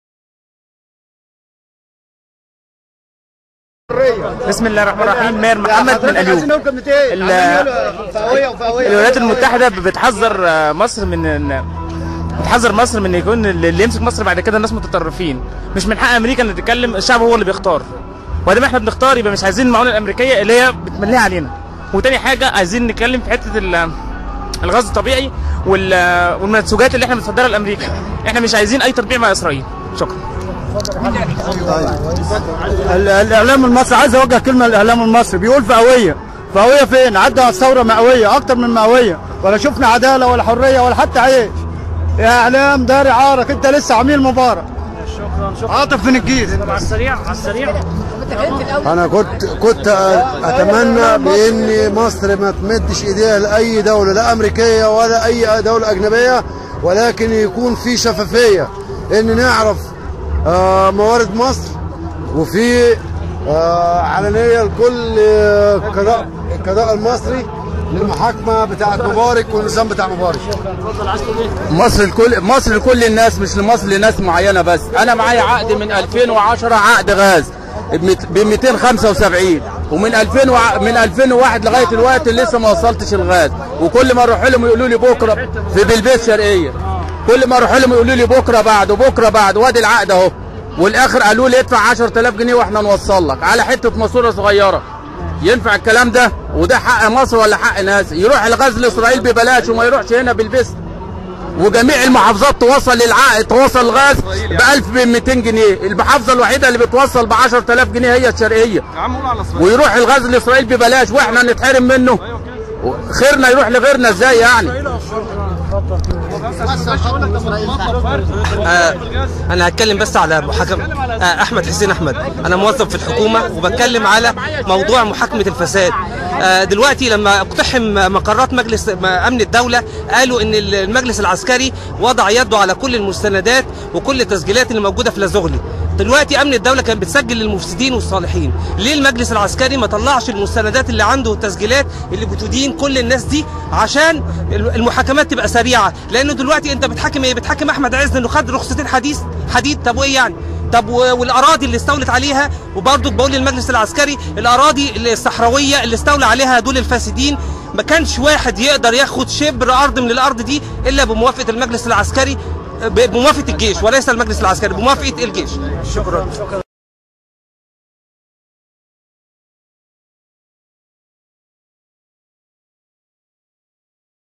مطالب المتظاهريين فى جمعة الحسم من التحرير
المتظاهرون يعرضون مطالبهم فى جمعة الحسم من ميدان التحرير